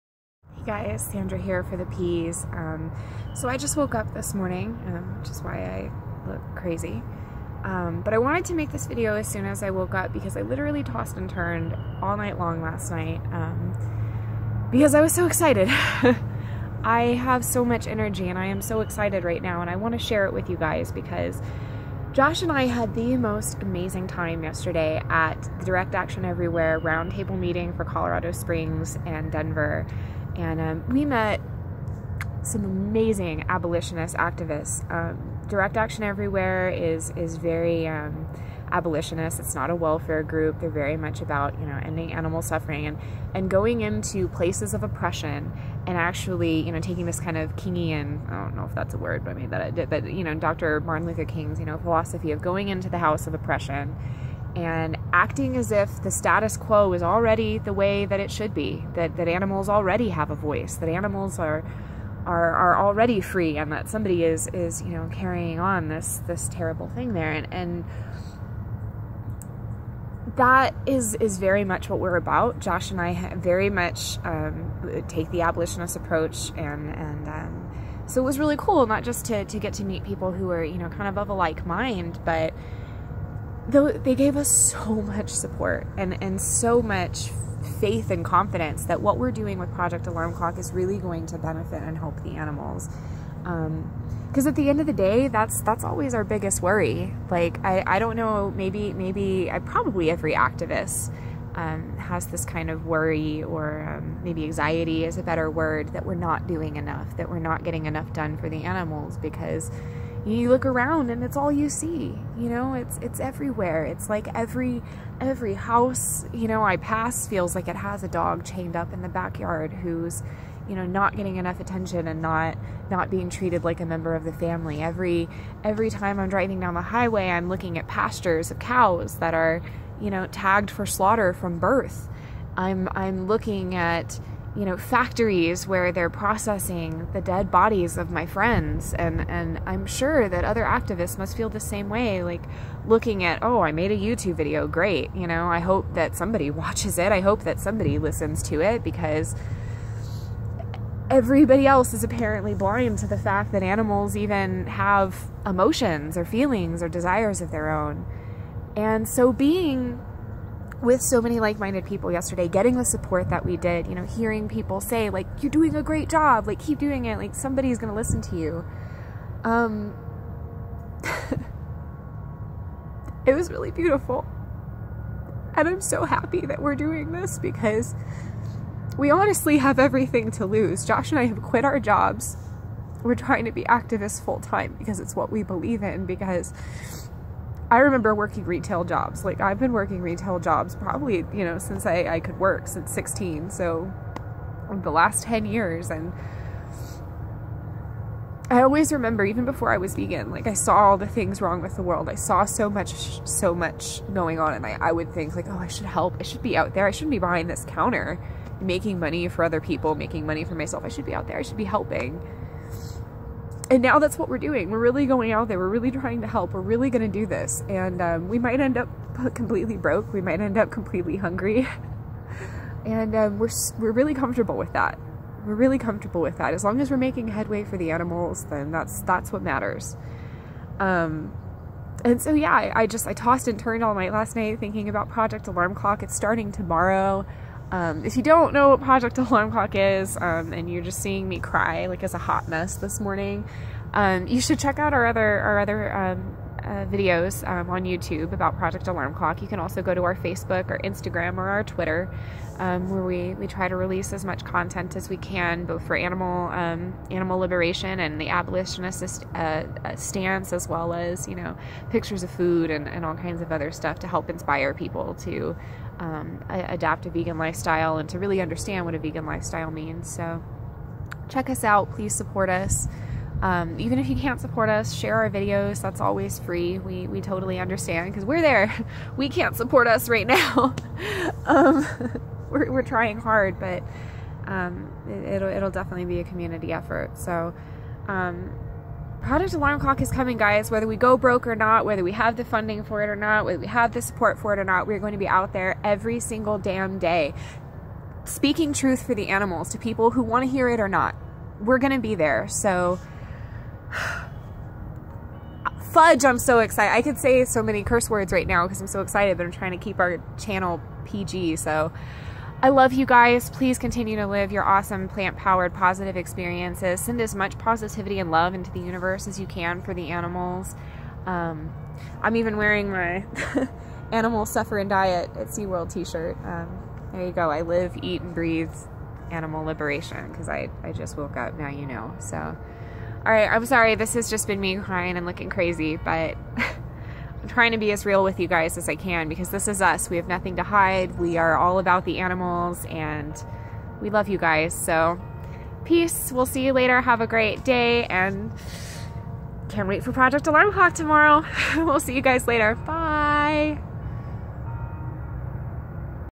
Split Peas: Give Peas A Chance
Sandrah may be looking crazy in this video but the message isn't crazy at all... ANIMALS DESERVE RESPECT AND PROTECTION. Please help us make that common knowledge.